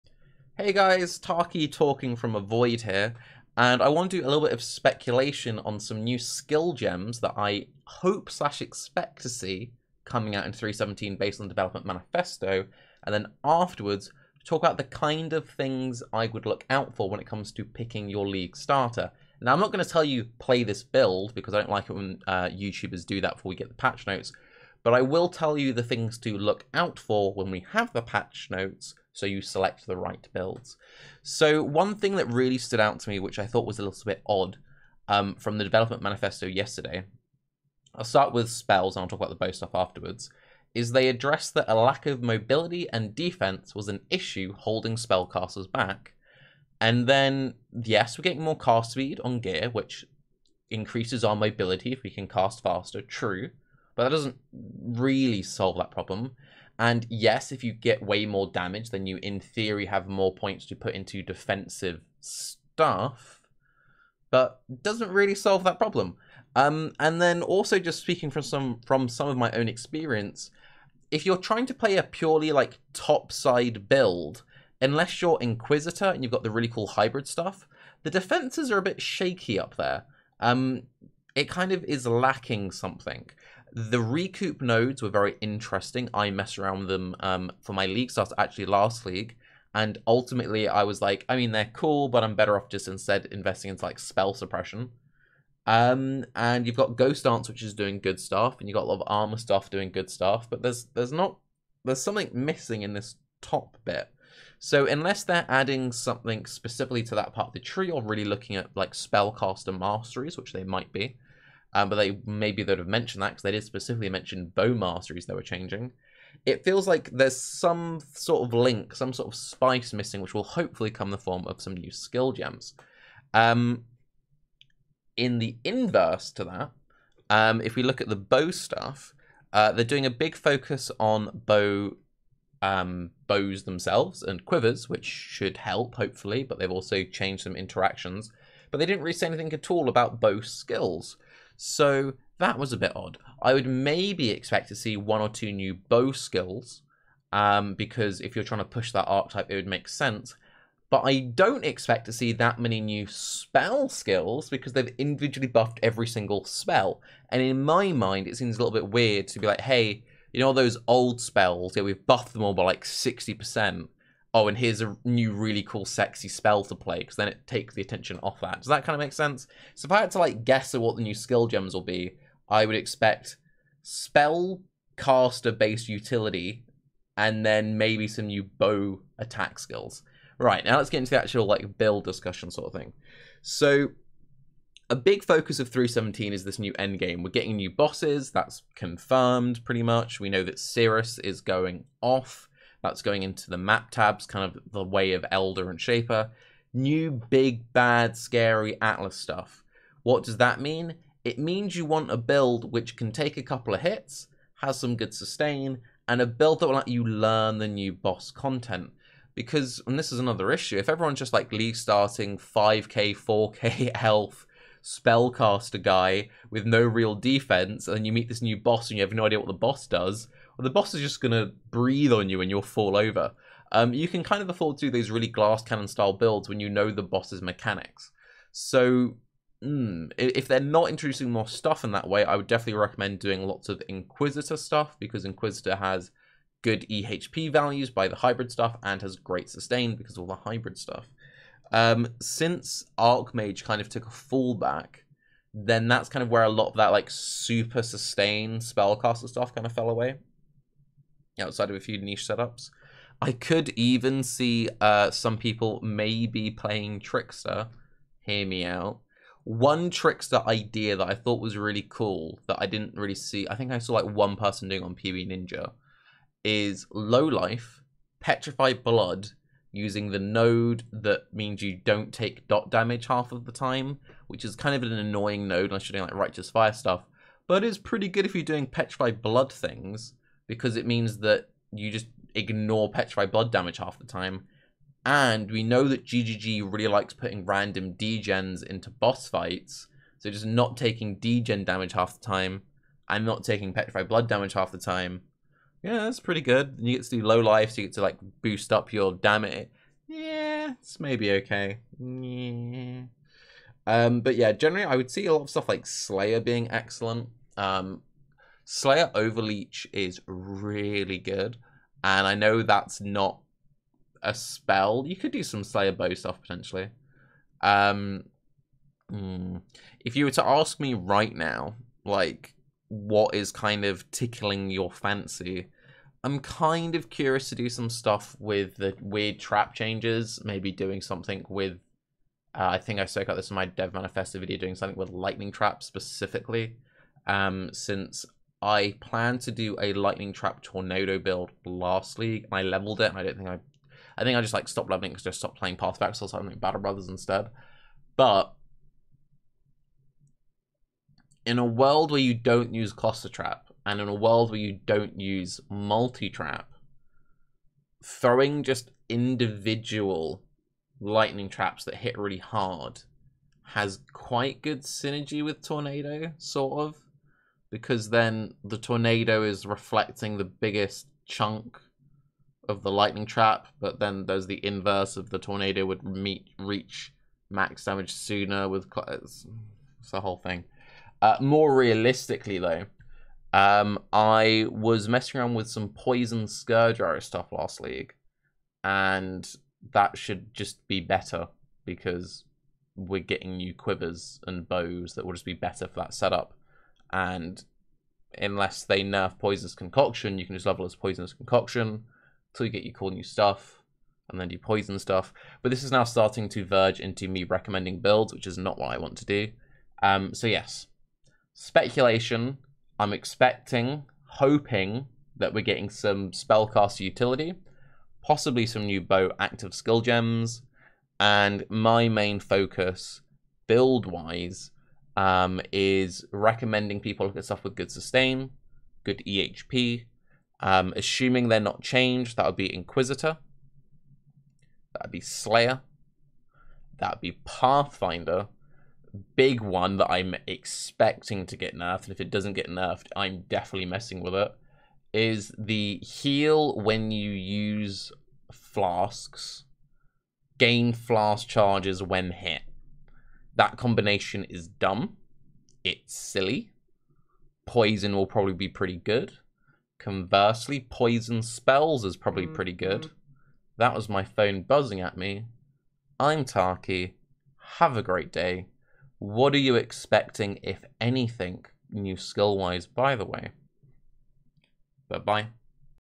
3.17 Builds & New Skills | Archnemesis Speculation
I've been thinking a lot about leaguestart builds "wow really Tarke?" So enjoy my speculation on the kinds of new skills we might see come 3.17 and what to avoid when it comes to picking your league starter.